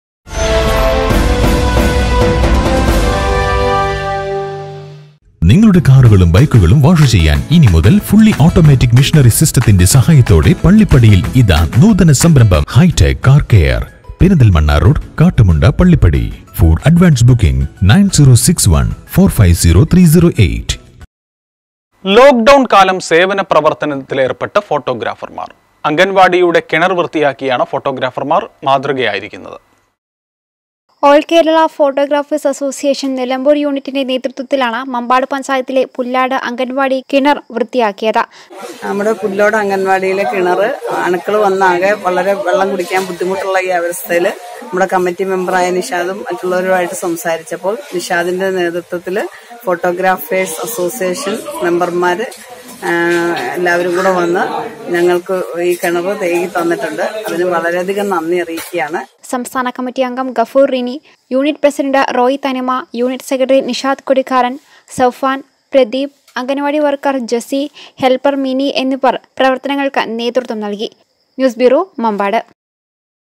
ONE TV NEWS
ലോക് ഡൗണ്‍ കാലം സേവന പ്രവര്‍ത്തനത്തിലേര്‍പ്പെട്ട് ഫോട്ടോഗ്രാഫര്‍മാര്‍. അംഗനവാടിയുടെ കിണര്‍ വൃത്തിയാക്കിയാണ് ഫോട്ടോഗ്രാഫര്‍മാര്‍
മാതൃക ആയിരിക്കുന്നത്.